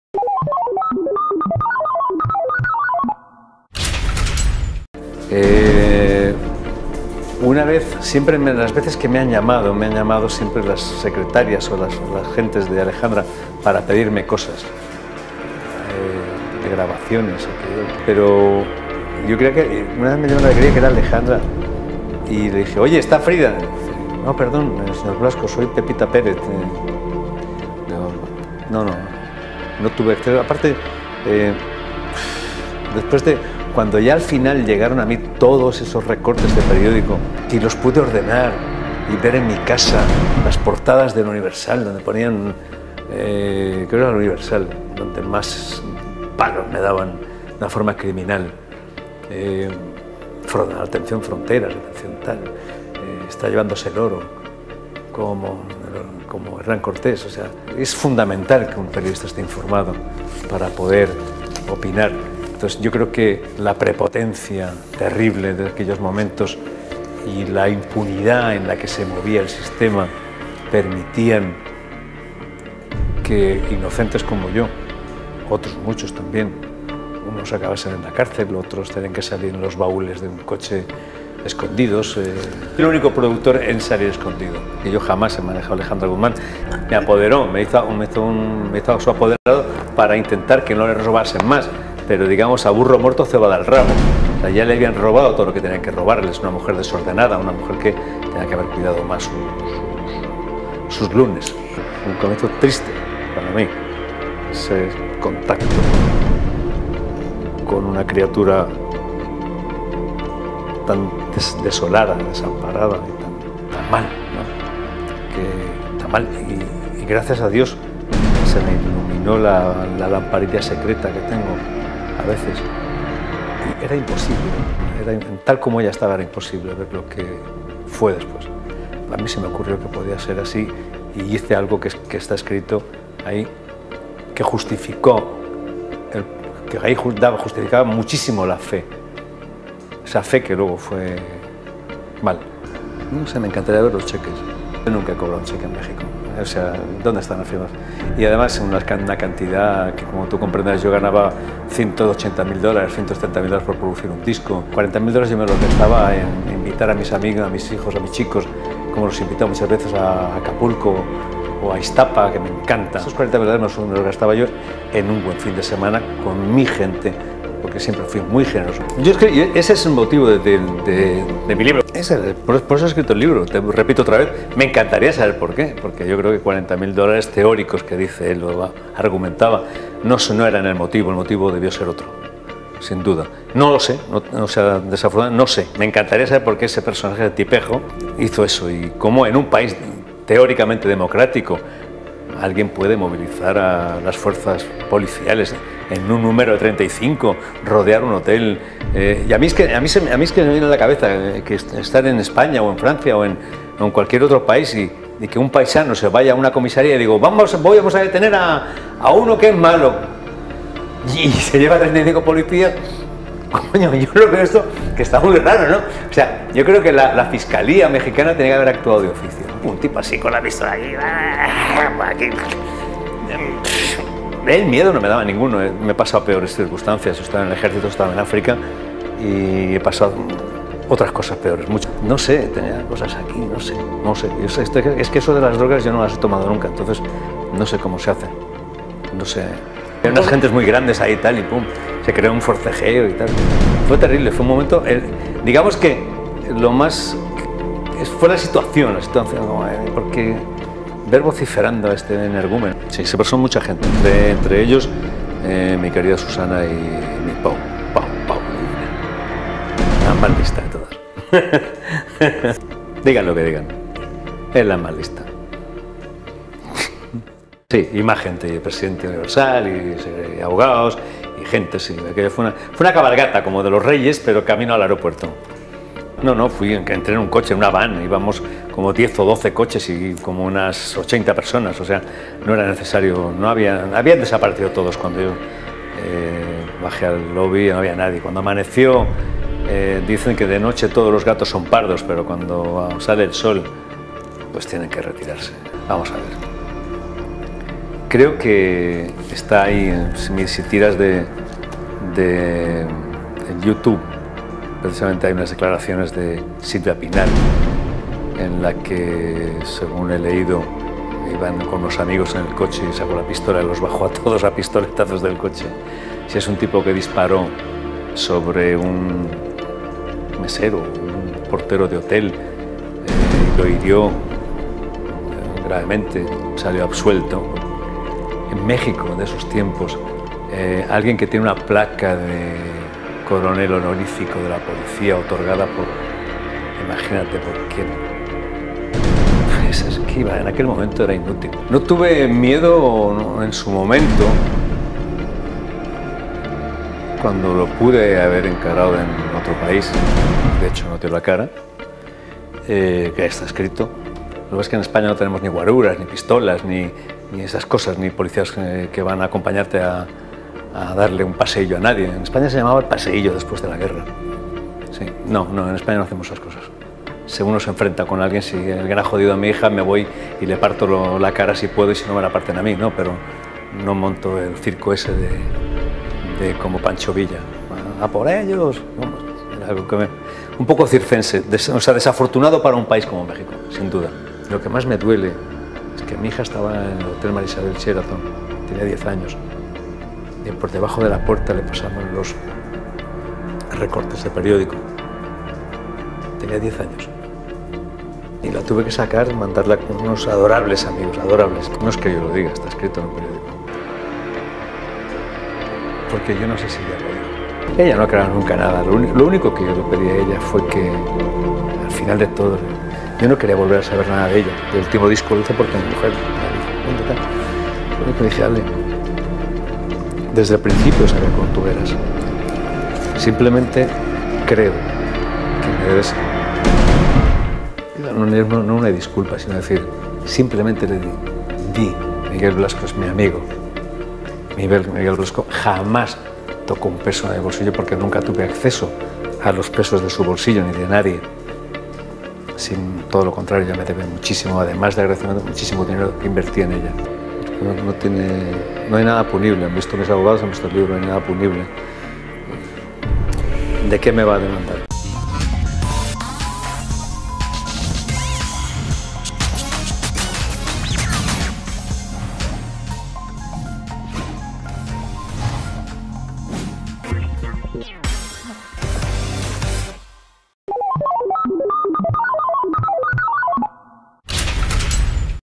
¡ MIGUEL BLASCO HABLA DE ALEJANDRA GUZMAN !
Hoy te presentamos, una entrevista con el productor musical Miguel Blasco, donde habla claro y fuerte sobre la serie de malas experiencias que tuvo al lado de la famosa Alejandra Guzmán.